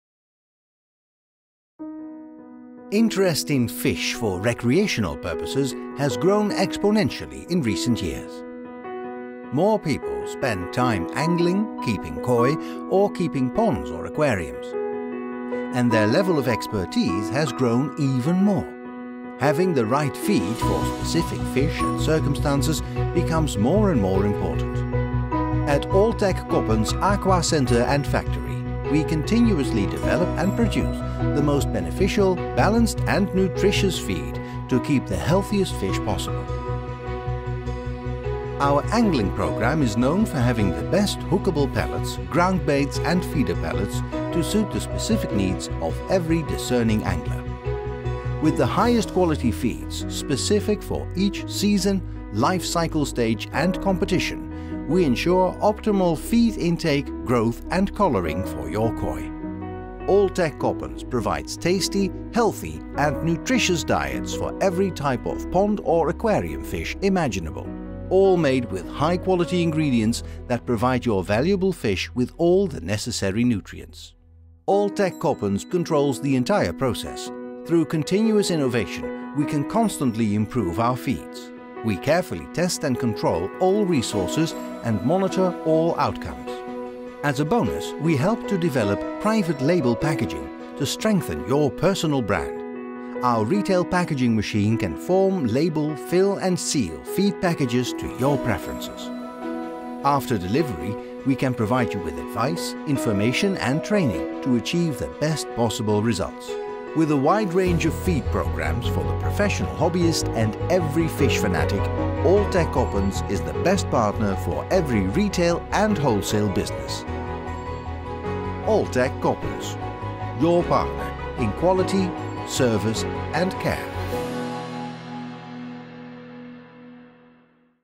Angling & Ornamental feeds
We have designed specific feed programs for koi, pond and aquarium fish. Our programs are designed for ornamental fish breeders and fish keepers, with a vast assortment that consists of premium quality fish feeds and specialist mixes for many types of ornamental fish.

From competition anglers who catch large numbers of small fish on light tackle to specimen hunters who fish for large carp and catfish with heavy-duty gear, we cater to every kind of angler. Our high-end angling program offers the discerning angler special extruded feed pellets, hookable baits and flavoured pellets. Each has been formulated to meet the specific needs of today’s anglers.